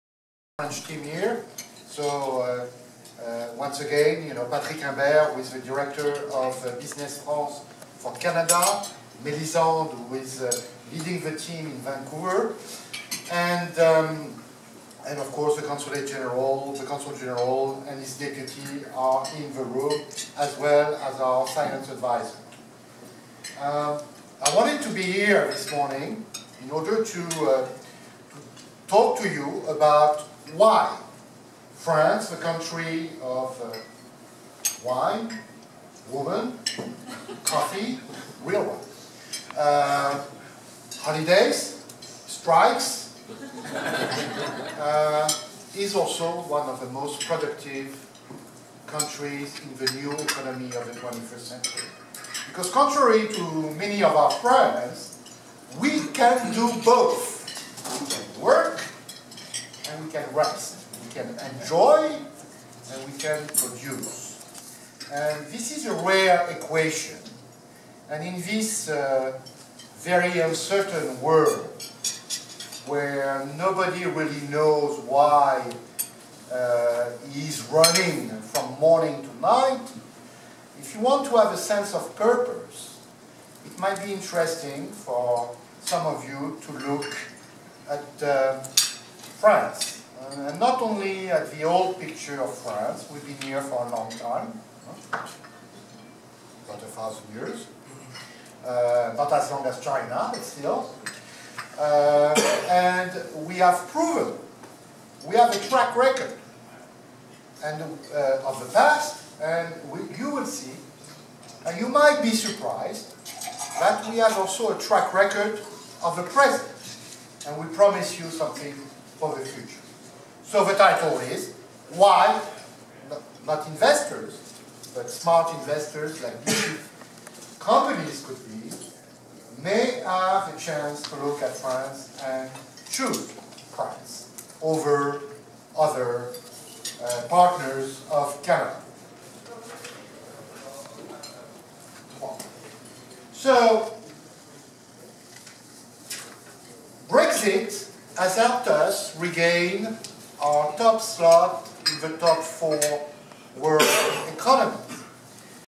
HE Ambassador of France Nicolas Chapuis speaks to Canadian companies
The signing of the EU-Canada Comprehensive Economic and Trade Agreement (CETA) will open vast new opportunities for Canadian businesses in a market as large as 500 million consumers.
- position at the heart of Europe,
- extensive infrastructure,
- world class academic and research network,
- advanced technological industry,
France is well placed to work as a hub for Canadian investments in Europe.

The French government has recently adopted new initiatives to make it easier and faster to set up and run a business, and it has introduced substantial tax incentives for investment in research and development. Due to strong economic and cultural ties, France is also a natural gateway to the vibrant African markets.

HE Ambassador Nicolas Chapuis, along with Business France the National agency for Investment in France, spoke with Canadian entrepreneurs at a breakfast-roundtable. Canadian companies Hootsuite and Sirona Biochem shared their experience doing business in France.